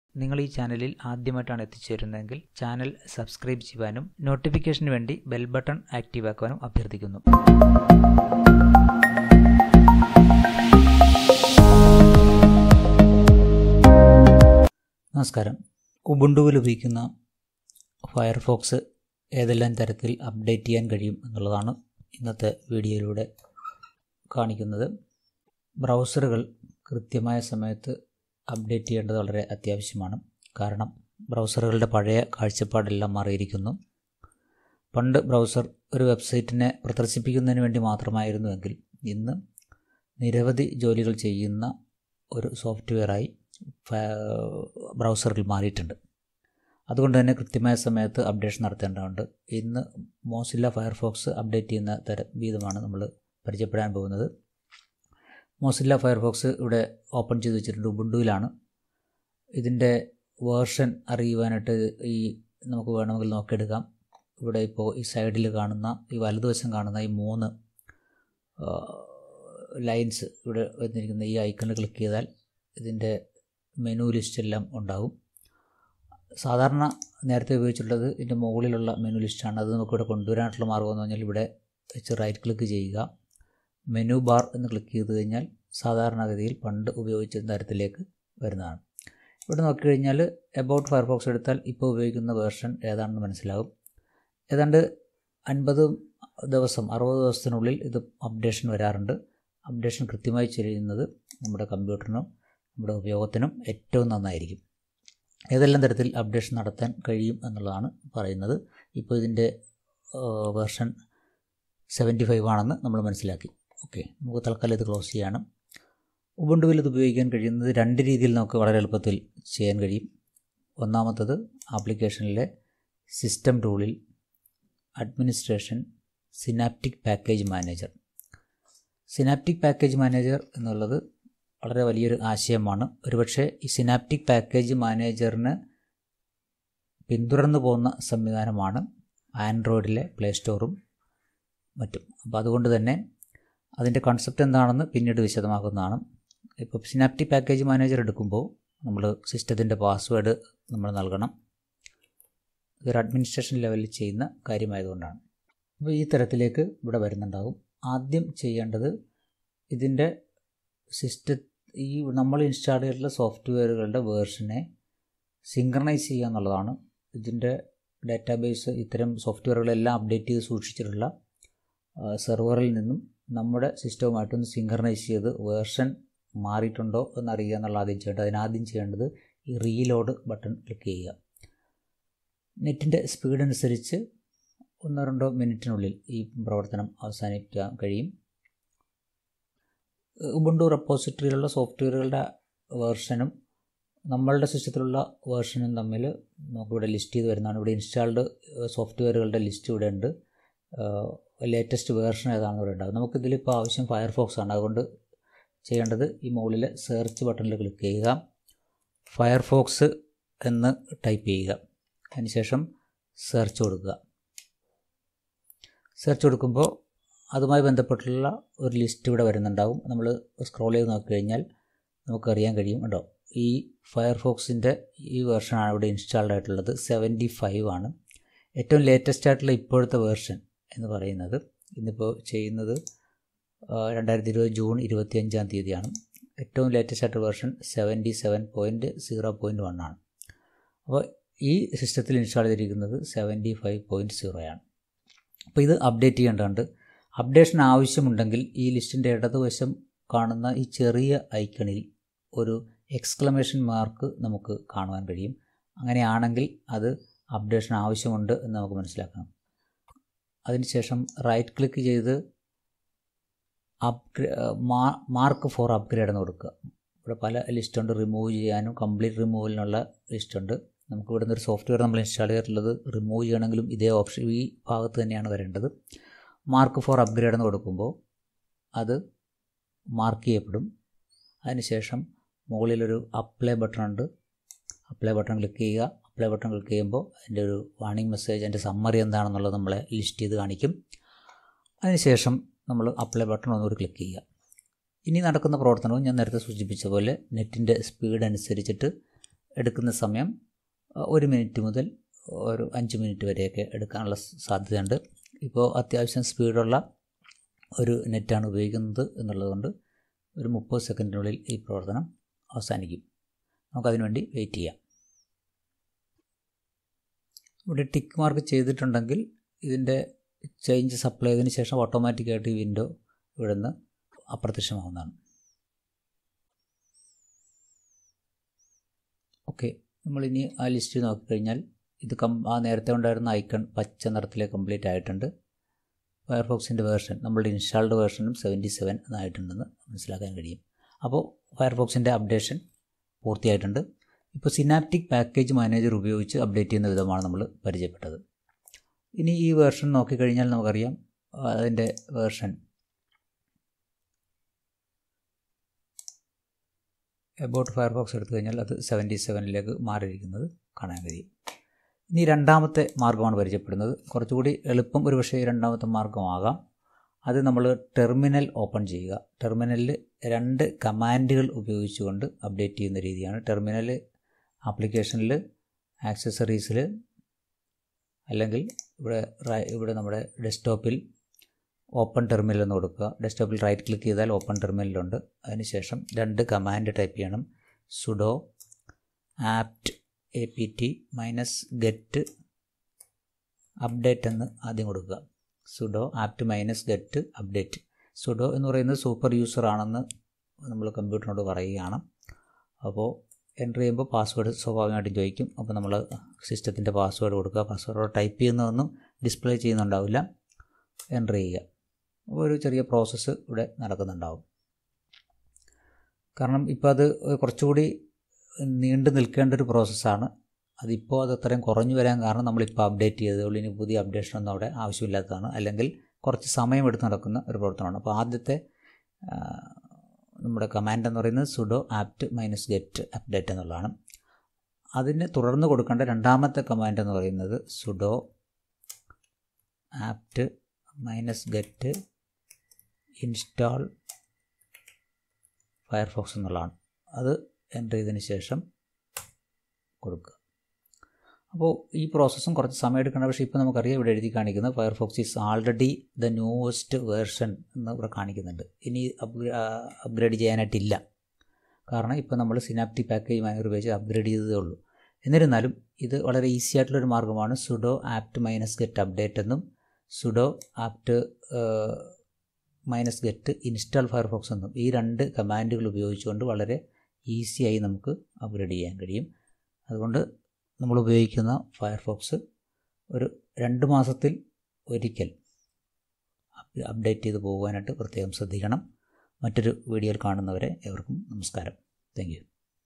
How to update Mozilla Firefox in Ubuntu | Firefox updation |Explained in Malayalam
This Video explains about how to update the browser Mozilla Firefox in Ubuntu. Two easy ways. Explained in Malayalam

For More Malayalam tutorial use the following links

Ubuntu settings Part 1

Ubuntu settings Part2